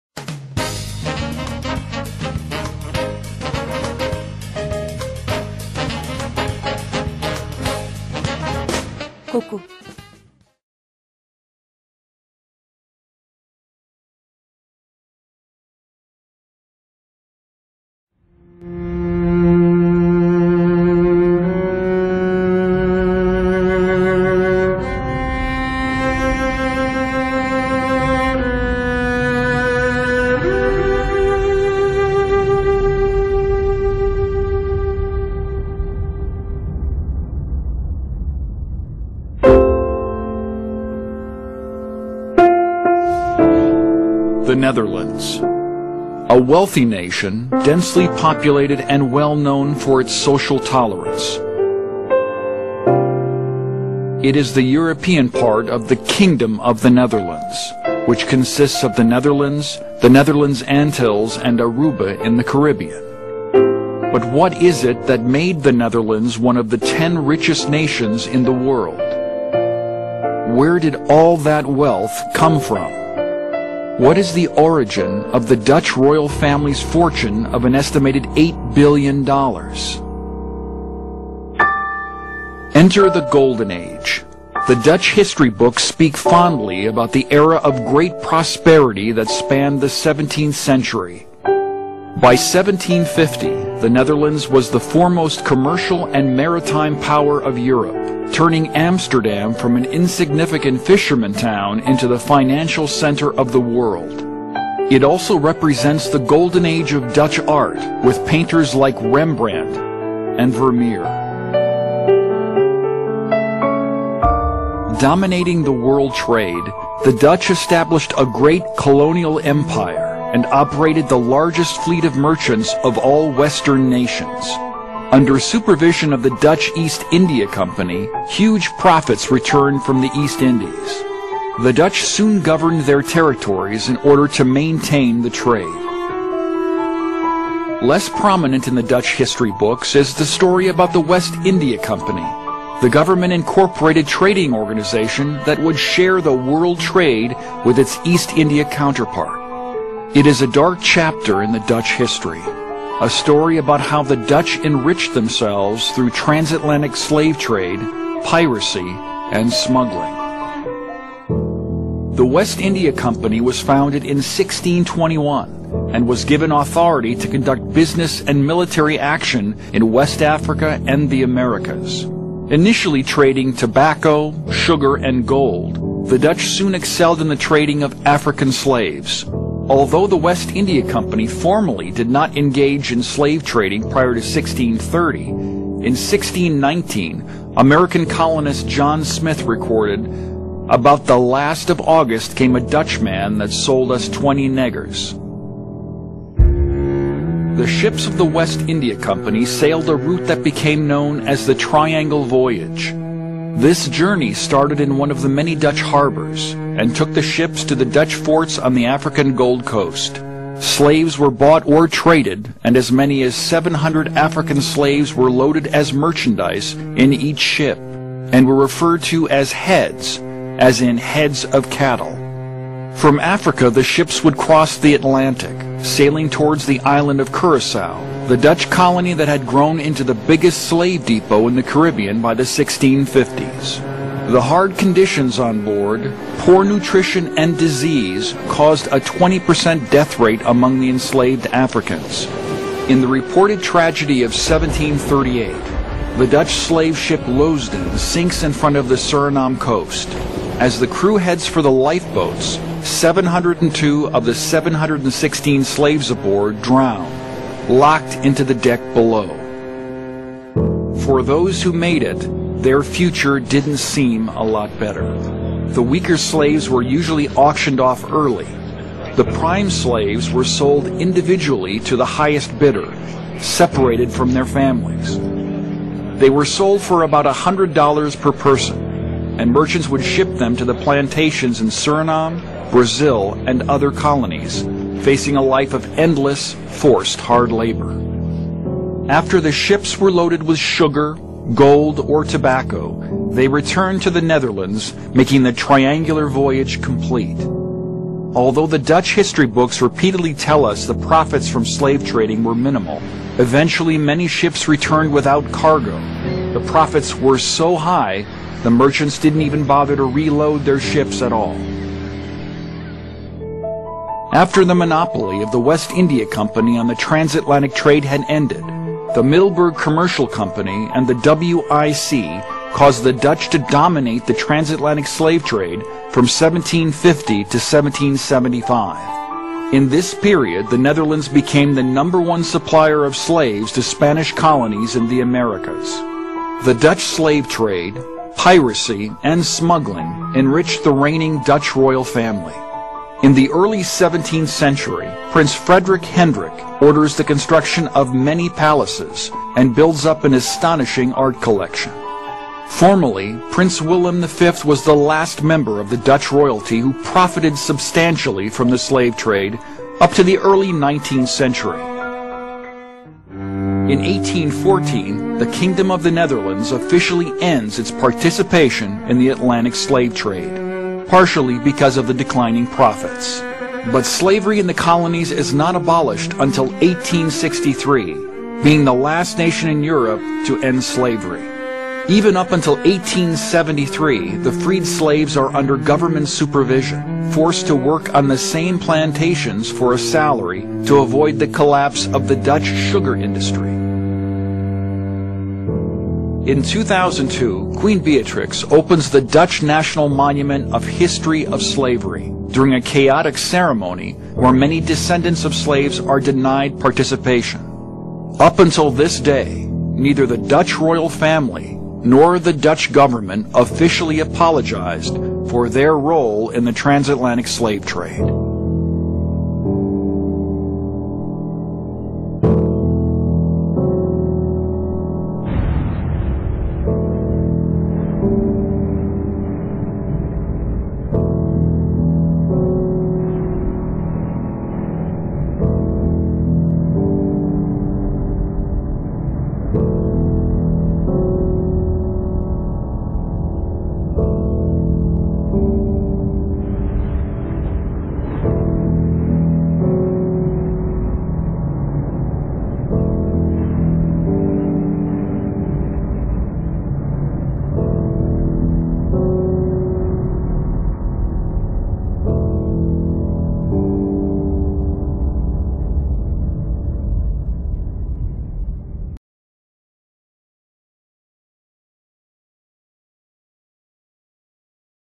Dutch West India Company; GWIC (Geoctroyeerde Westindische Compagnie)
TÜRKÇE: (scroll down for the English text) 1600lü yıllarla beraber, dünya ticaretinde Hollandalıların etkisi inanılmaz oldu. Örgütlü bir sermaye piyasası desteğinde baharat ticaretini ellerine geçirip fiyatlarla istedikleri gibi oynadılar. Biz daha çok baharat ticareti yapan "Doğu Hindistan Şirketi" (VOC) üzerine konuşmamıza rağmen, mevcut tek şirket bu değildi ve bir de "Batı Hindistan Şirketi" (Geoctroyeerde Westindische Compagnie, yani "GWIC")  vardı. Bu şirkette kraliyet ailesinin de hissesi vardı ve Karayipler, Brezilya ve Kuzey Amerika bölgesinde ticaret yapmak üzere yetkilendirilmişti. İngilizlere geçince New York adını alan New Amsterdam gibi kolonileri hep bu şirket kurmuştu. Bu şirket var olduğu süre içinde yoğun ve acımasızca köle ticareti (ve korsanlık) yaptı ve büyük paralar kazandı.

Bu şirketin de ortağı olan Hollanda Kraliyet Ailesi'nin servetinin bugün 8 milyar dolar seviyesinde olduğu söyleniyor.

Kokudan gelen ve kokuya giden şeylerle ilgili daha fazla bilgi için lütfen adresini ziyaret ediniz.

ENGLISH: Starting with the 1600s, the Dutch had an unbelievable impact on world trade. With the help of an organised financial system, they controlled the whole spice trade and manipulated the prices as they wished. We have talked about the East India Company, V.O.C., which traded in spices mostly, but this was not the only corporation that existed and there was also the West India Company (Geoctroyeerde Westindische Compagnie, or "GWIC" in short). This corporation, at which the royal family was also a shareholder, was authorised to trade in the Caribbean, Brasil and North America. Colonies such as New Amsterdam (which became New York when left to the British) were all formed by this company. Throughout its existence, this corporation was involved in immense( and ruthless) slave trading, along with pirating and made big fortunes.

The Royal Dutch Family who was a partner of this corporation is said to have around 8 billion dollars of wealth today.

For more information about everything related to the sense of smell, please